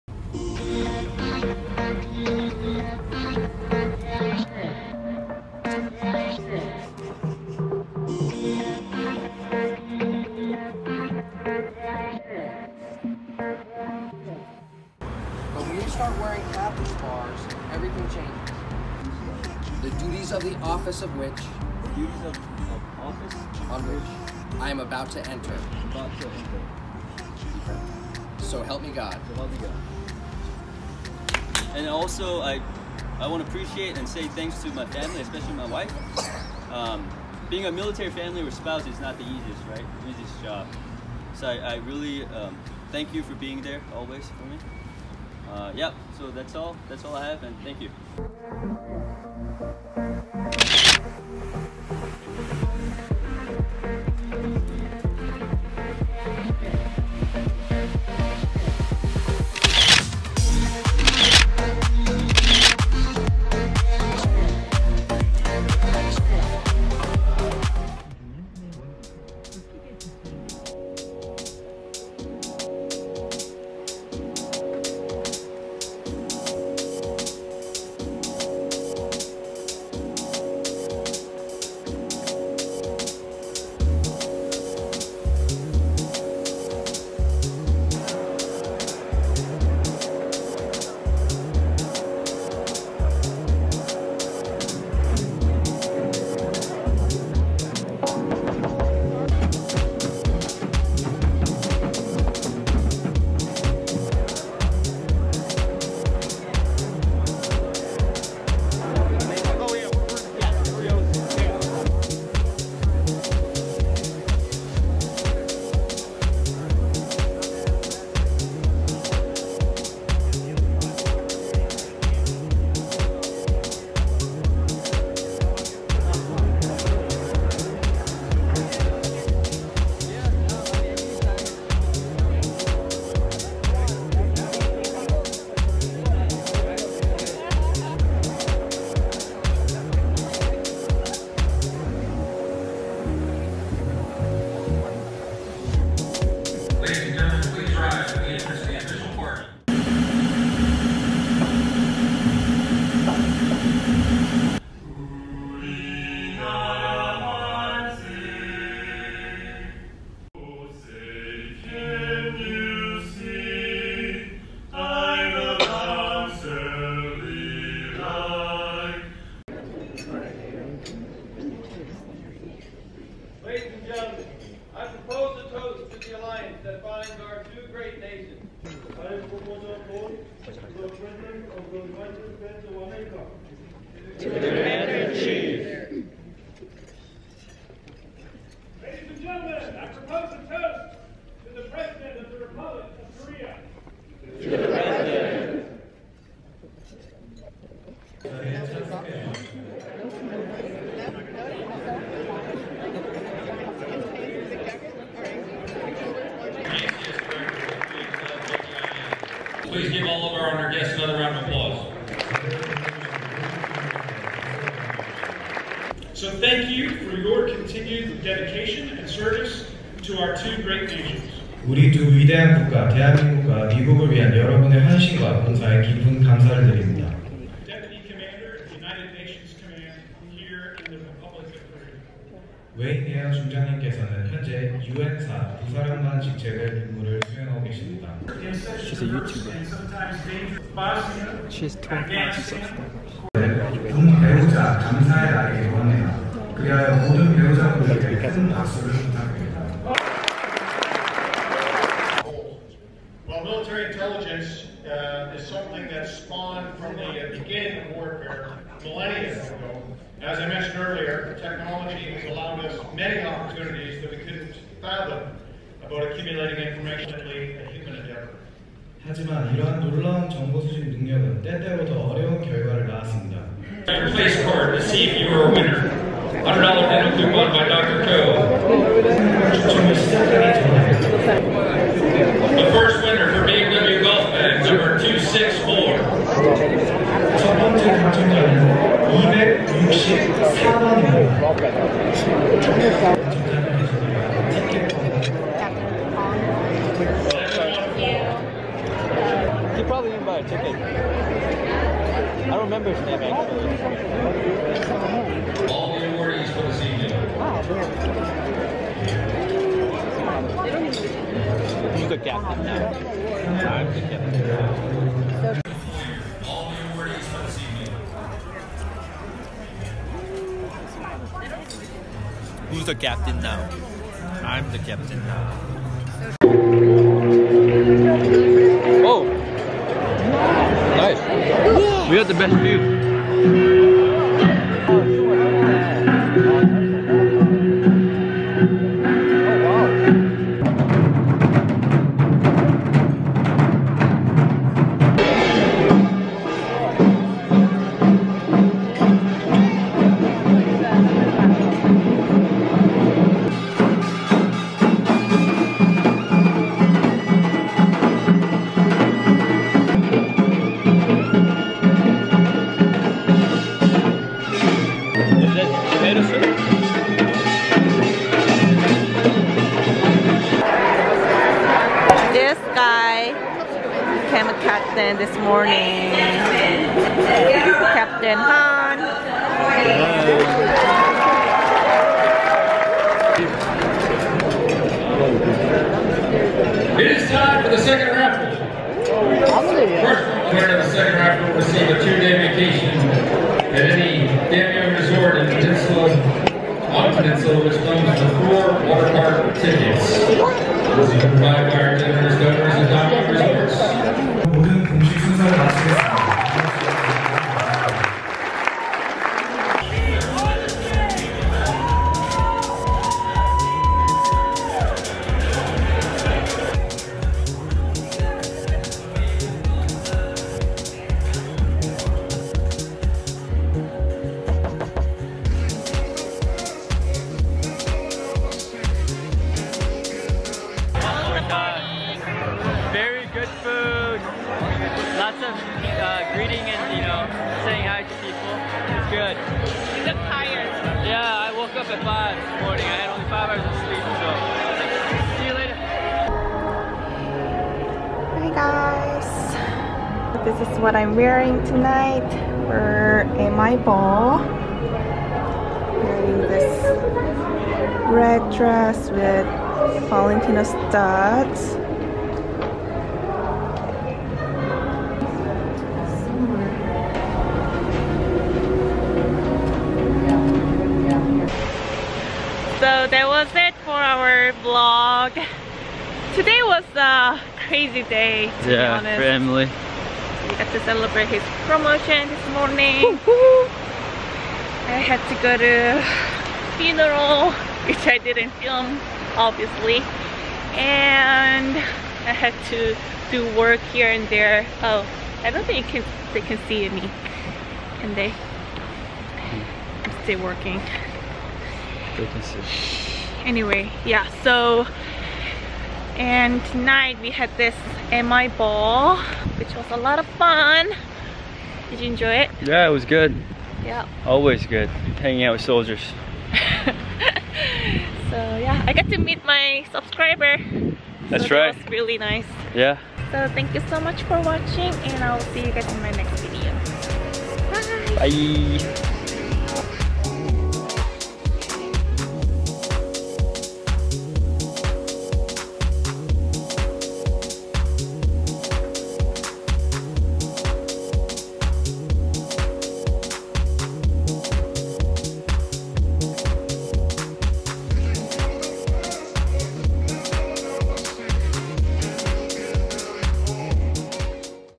[VLOG] CAPTAIN PROMOTION + M.I. BALL | 남편 대위 진급식 + 미군 파티 | GOONINWIFE
Hi guys,
Recently, my husband was promoted as a captain and we attended M.I. Ball on the same day. Please congratulate my husband for his promotion! :)

안녕하세요?
최근에 제 남편이 대위로 진급했어요!! :) 그리고 같은 날 미군 파티에도 참석했는데요, 영상 재밌게 봐주시고 '좋아요'와 '구독'버튼도 꼭 눌러주세요! :)